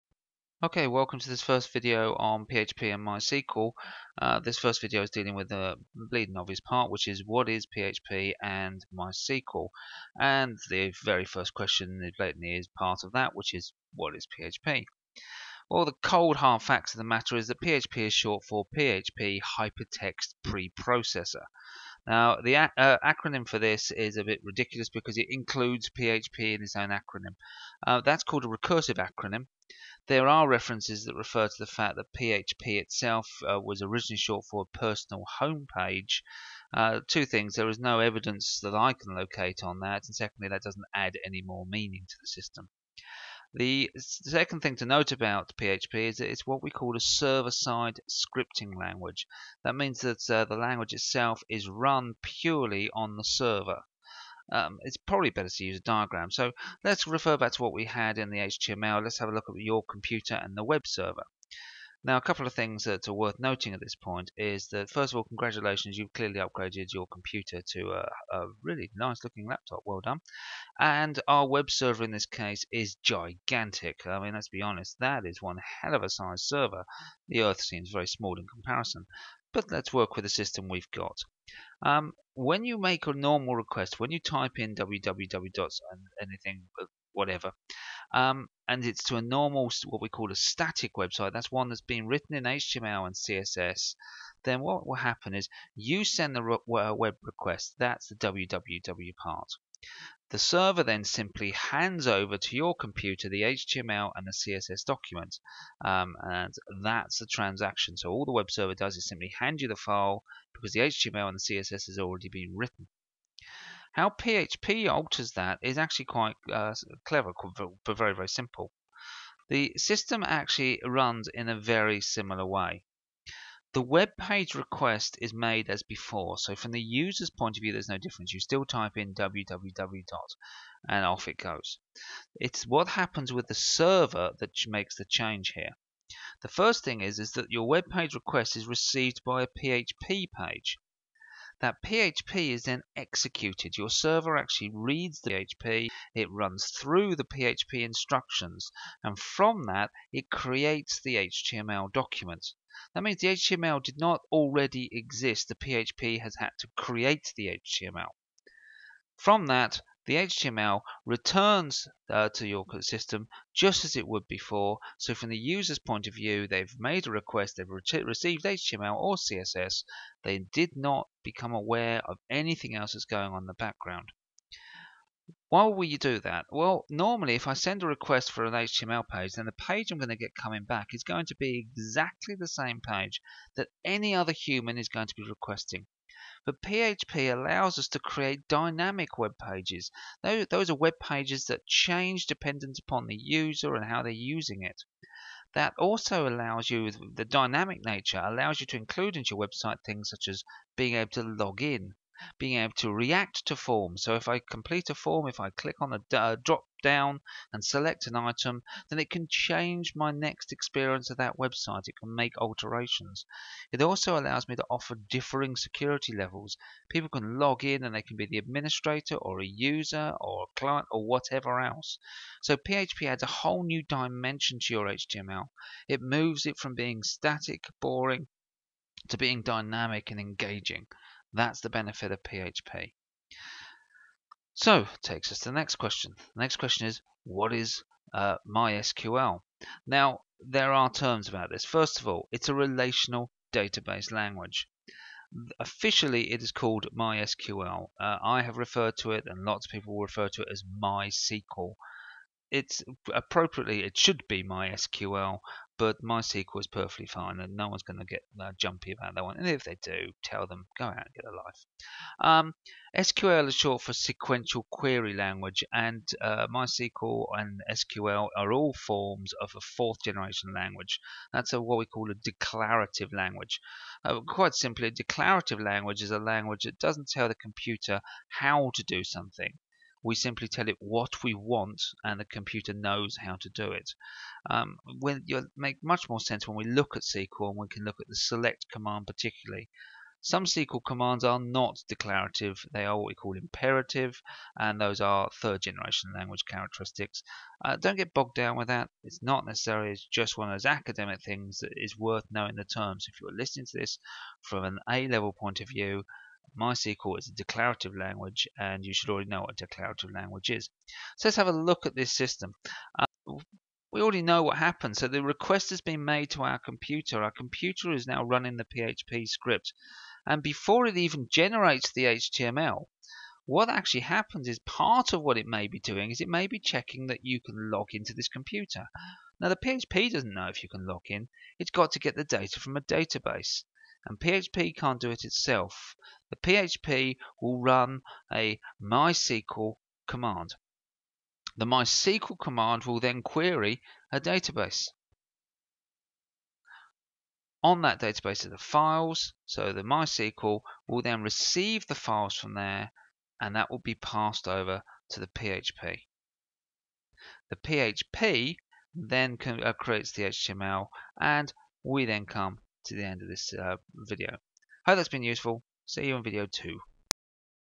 PHP and MySQL 1 - What is PHP and MySQL?
Discussing the terms PHP and MySQL; defining their purpose within a web-based system.